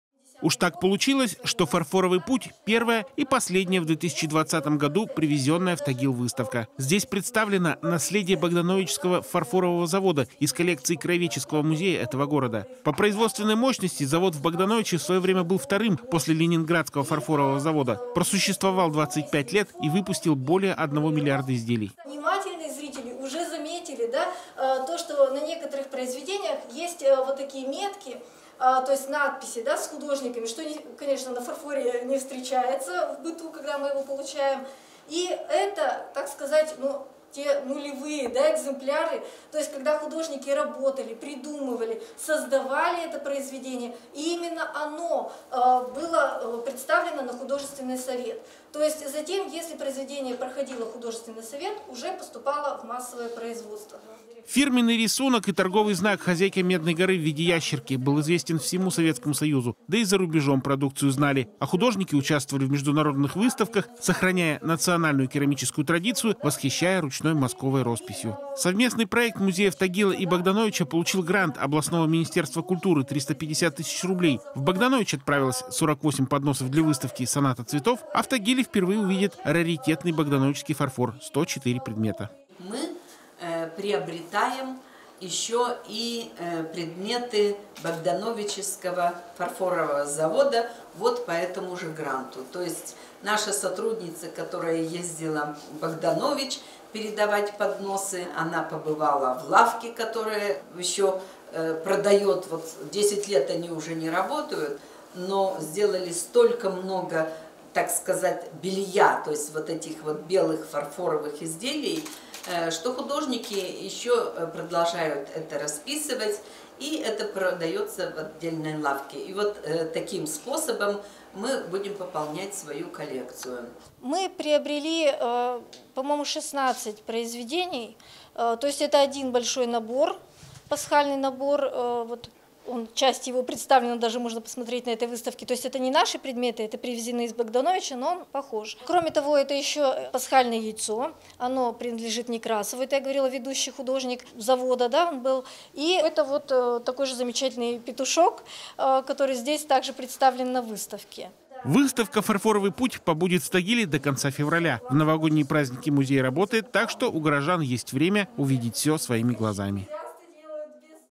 «Фарфоровый путь»
Первая и последняя выставка в 2020 году привезенная в Нижний Тагил. В музее изобразительных искусств открылась экспозиция «Фарфоровый путь», в ней представлено более сотни экземпляров раритетного богдановического фарфора. В свою очередь музей искусств отправил в Богданович выставку подносов «Соната цветов». Обменные экспозиции реализованы на средства гранта Министерства культуры Свердловской области.